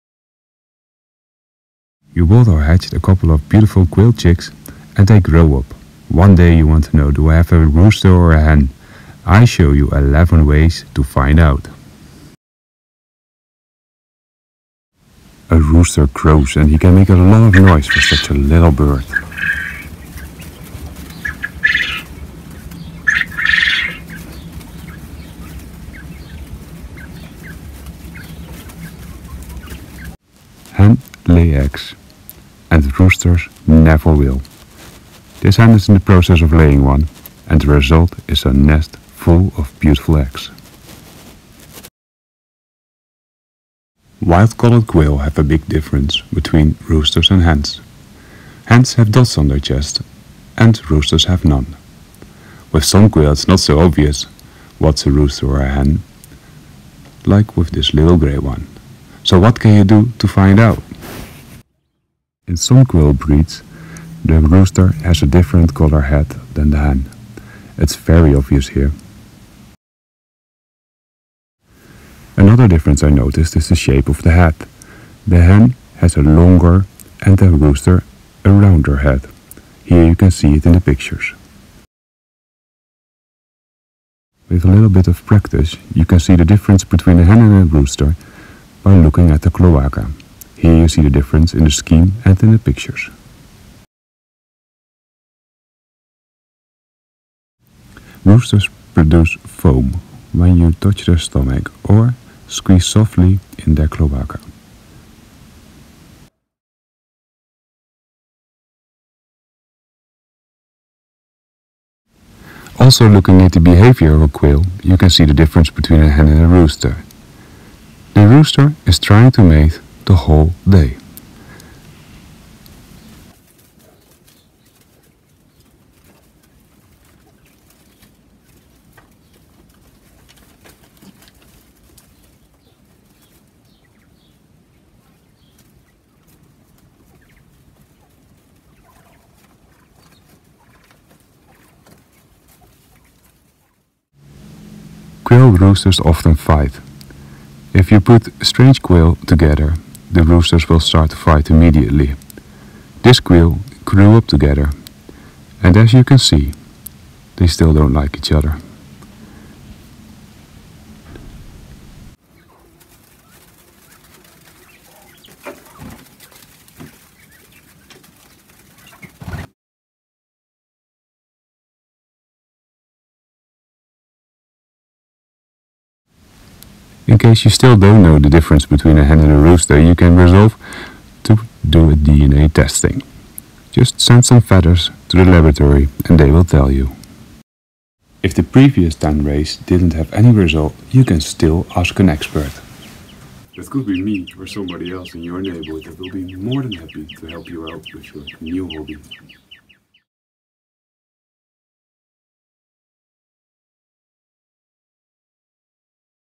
11 ways to find out if your quail is a hen or a rooster
An explanation how to see the difference between a hen and rooster quail. A guide for beginner quail breeders. Use a combination of this 11 ways to find out to know for sure.